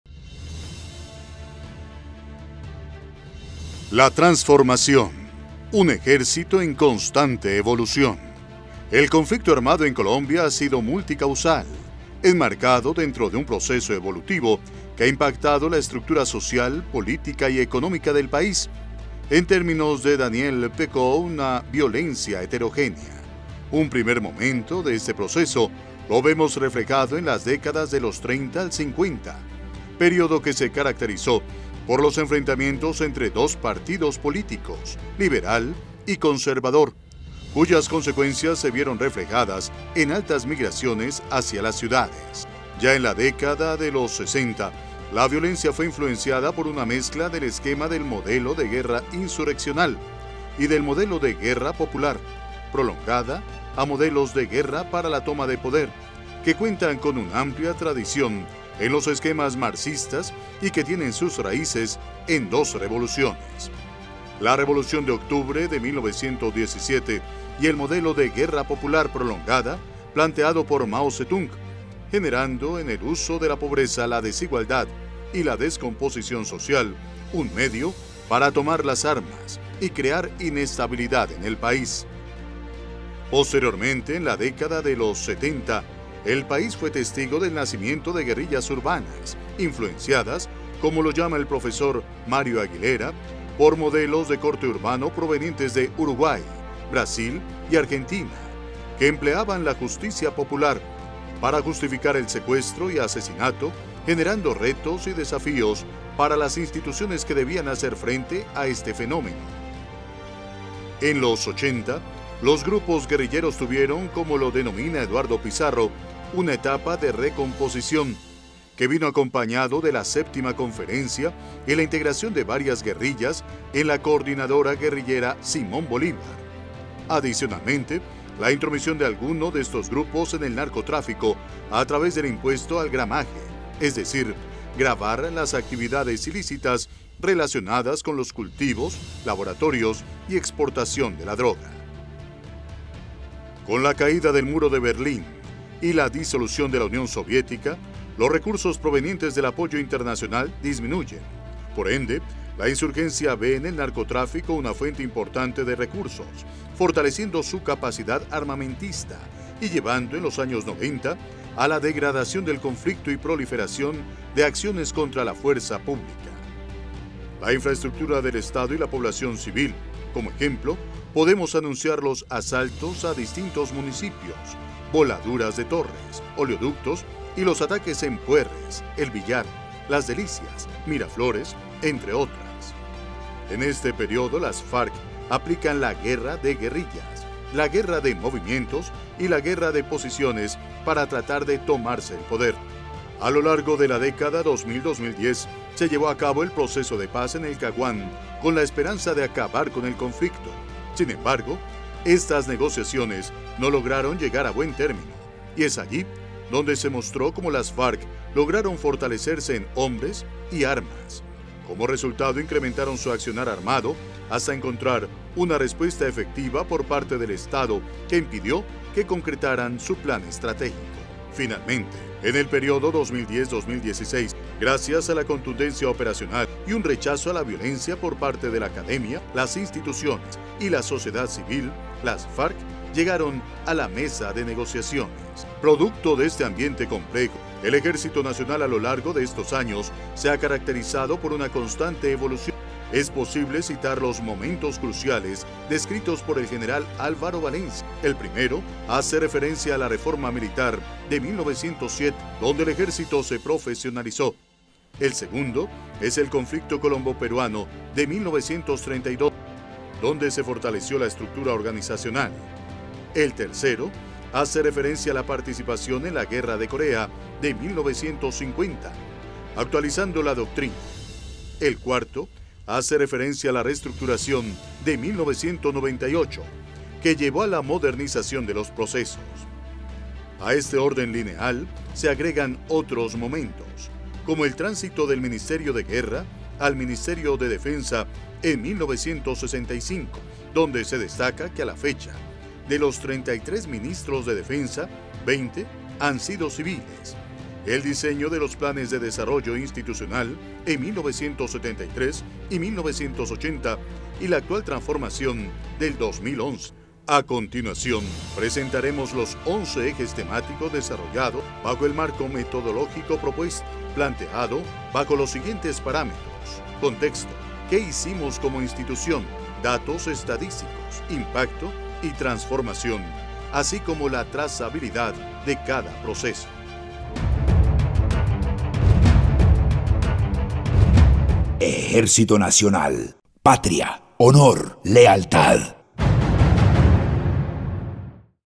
Ejército Nacional, 50 años de transformación
Es una narración rigurosa, en 11 ejes temáticos, que cuenta la historia y transformación del Ejército Nacional, desde 1963 hasta el año 2022. En estas piezas audiovisuales se narra la trayectoria de la institución desde el proceso de evolución, los avances profundos implementados en todos los niveles: estratégico, operacional y táctico, evidenciando la adaptación institucional a los escenarios con la misma velocidad con la que evoluciona su entorno.

El conflicto armado en Colombia ha sido multicausal, enmarcado en un proceso evolutivo que ha impactado la estructura social, política y económica del país, en términos de Daniel Pecaut, una violencia heterogénea. Revisemos entonces los momentos de este proceso.